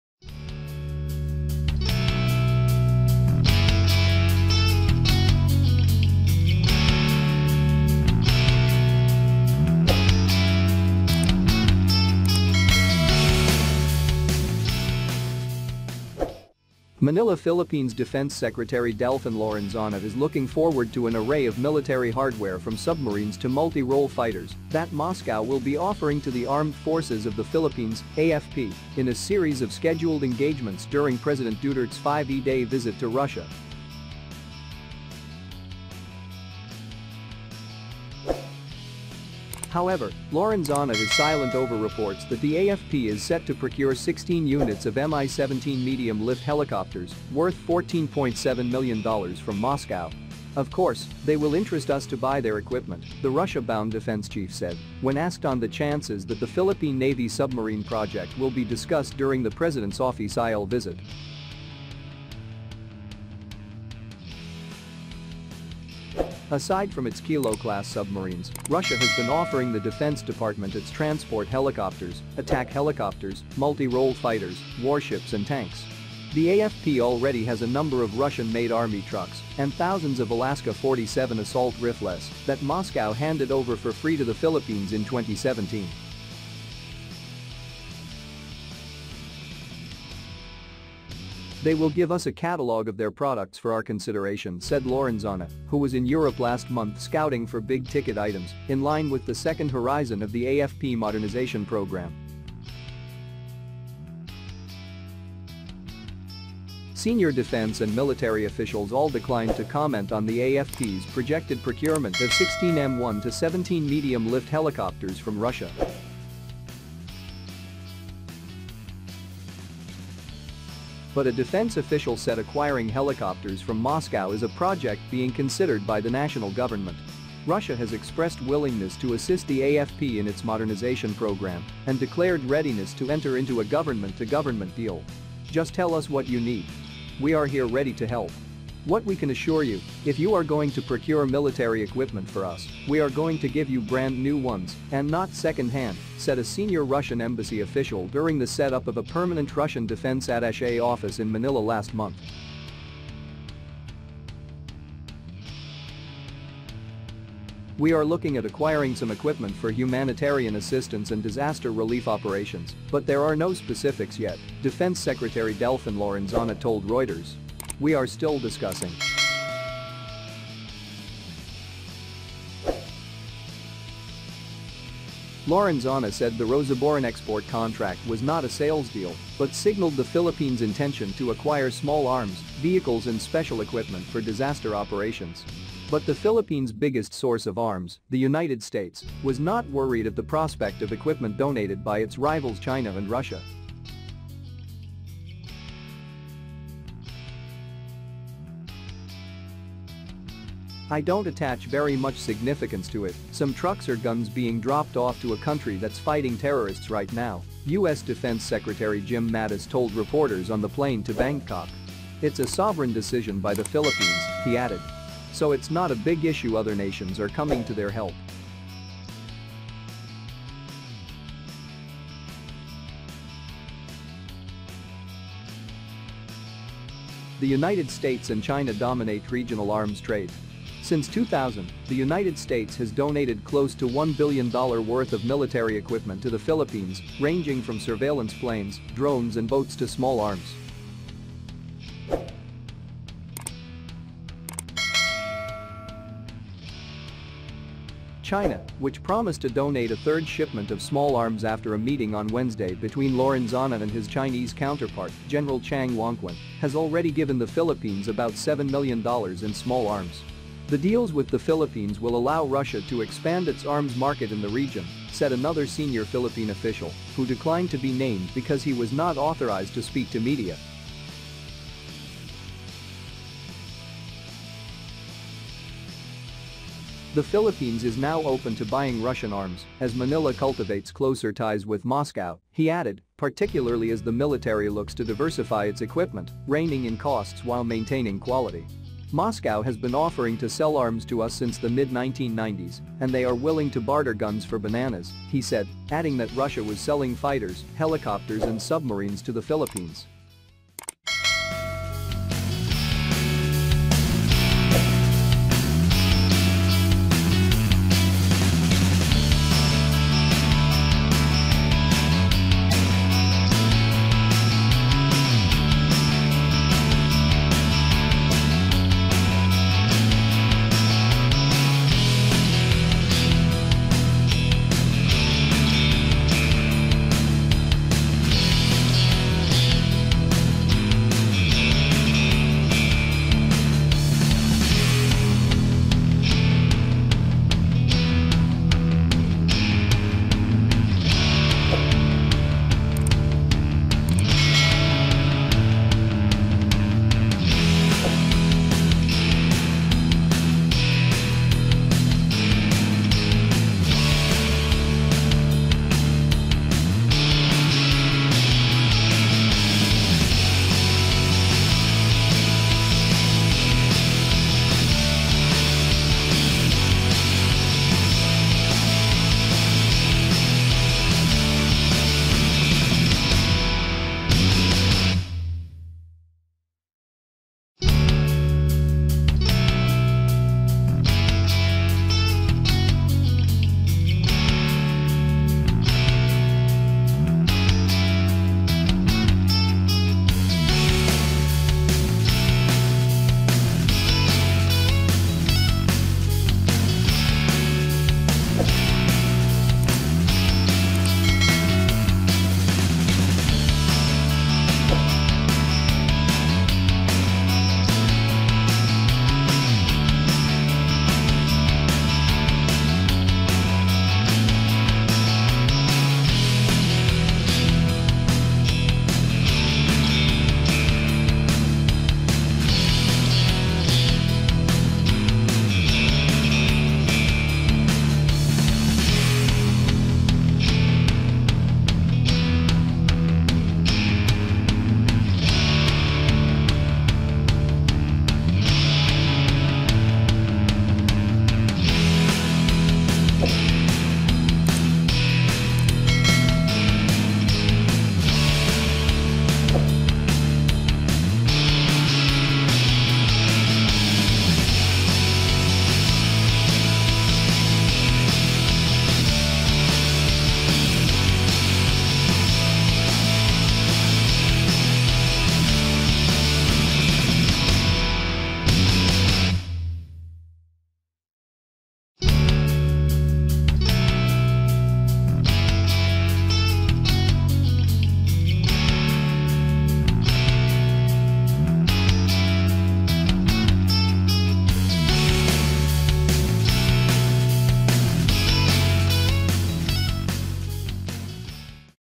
Philippine Ready : Armed Forces of The Philippines AFP Eyes Military Hardware from Russia
MANILA, Philippines — Defense Secretary Delfin Lorenzana is looking forward to an array of military hardware – from submarines to multi-role fighters – that Moscow will be offering to the Armed Forces of the Philippines (AFP) in a series of scheduled engagements during President Duterte’s  fi ve day visit to Russia.

“Of course, they will interest us to buy their equipment,” the Russia-bound defense chief said when asked on the chances that the Philippine Navy submarine project will be discussed during the President’s offi cial visit.